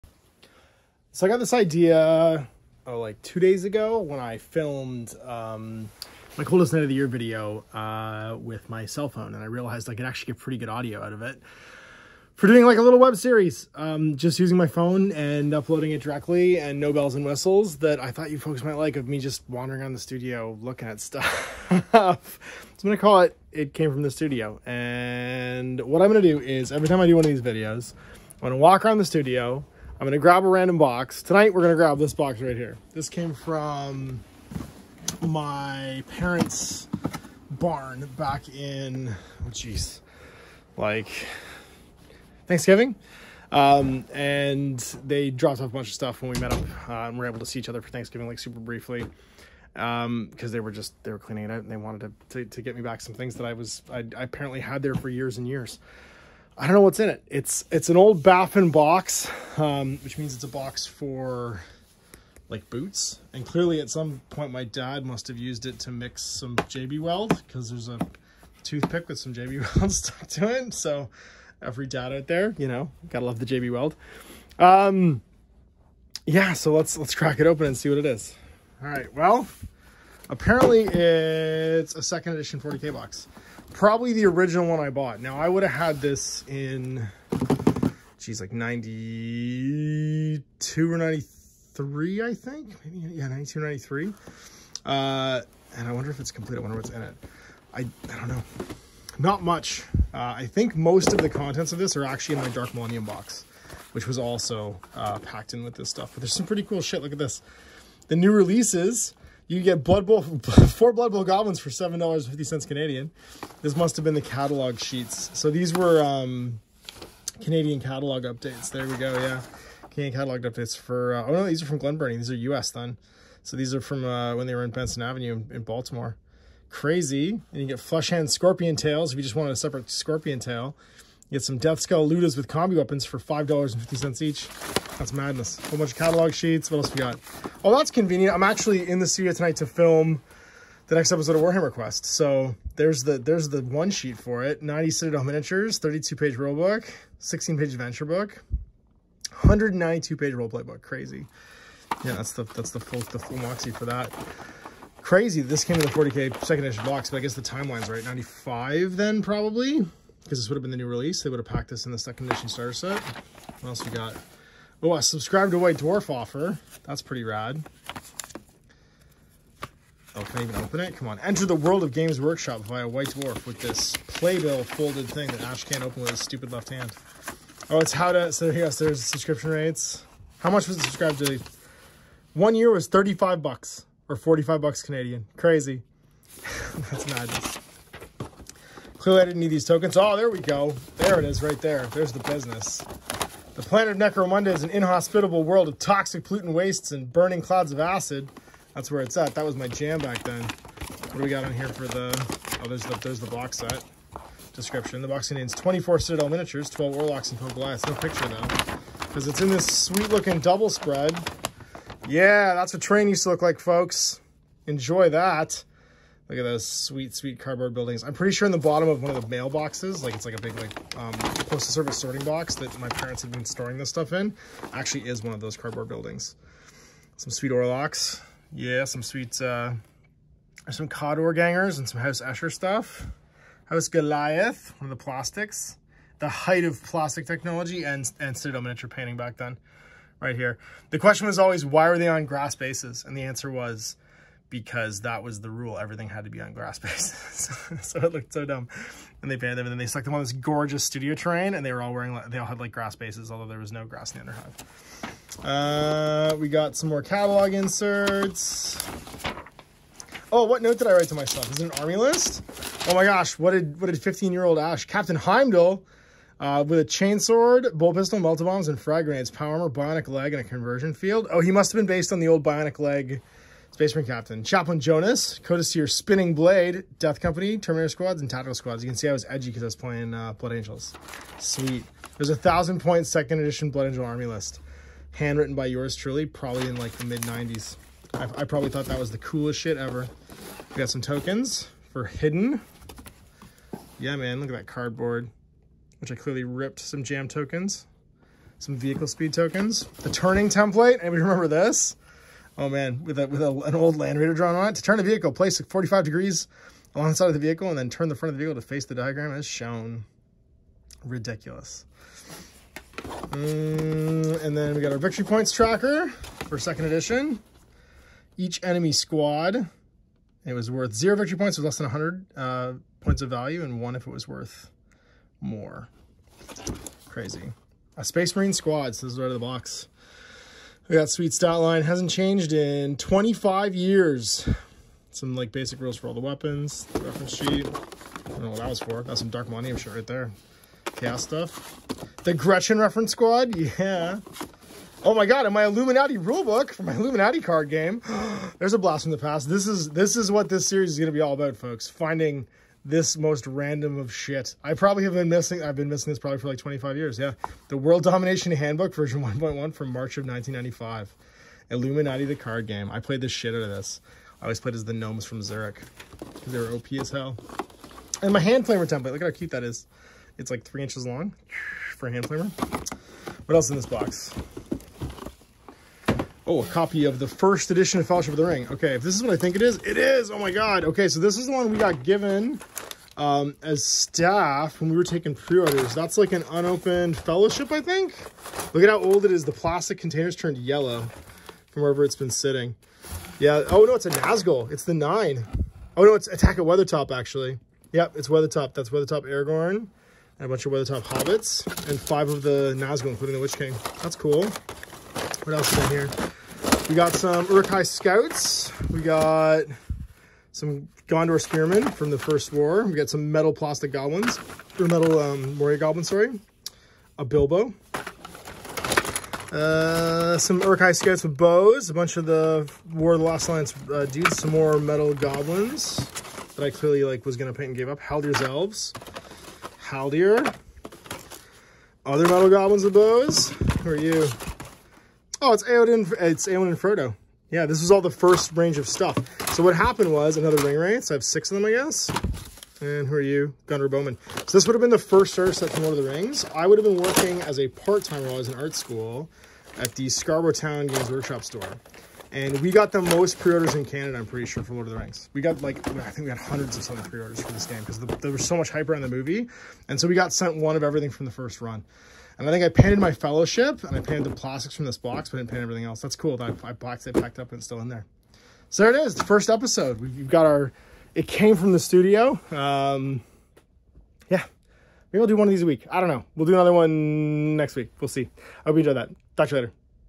It Came from the Studio Ep 01
Ever wonder where what kind of things lurk in the corners of my studio or deep in the darkest depths of my storage unit? Me too! I’ll throw the camera on once a week and open up boxes in the hopes that it gets me organized!